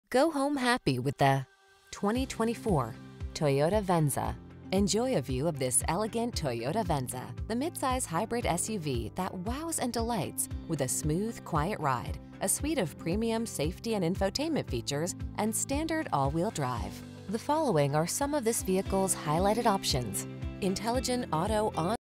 2024 Toyota Venza LE VT Bellows Falls, Vermonat at Durand Toyota Ford. Servicing the Keene, Bra...
New 2024 Toyota Venza LE available at Faiths Toyota Ford located in 6896 US-5, Westminster, VT, 05158. Servicing the Bellows Falls, Vermonat at Durand Toyota Ford. Servicing the Keene, Brattleboro, Claremont, Greenfield area.

Go home happy with the  2024  Toyota Venza. Enjoy a view of this elegant Toyota Venza, the midsize hybrid SUV that wows and delights with a smooth, quiet ride, a suite of premium safety and infotainment features, and standard all-wheel-drive.  The following are some of this vehicles highlighted options: Intelligent Auto on/off High Beams, Pre-Collision System, All Wheel Drive, Navigation System, 4 Cylinder Engine, Lane Keeping Assist, Back-Up Camera, Dual Zone A/C, Hybrid, Aluminum Wheels. Rediscover the pleasure of travel in this passenger-focused Venza. Come in for a test drive. Our team will make it the best part of your day.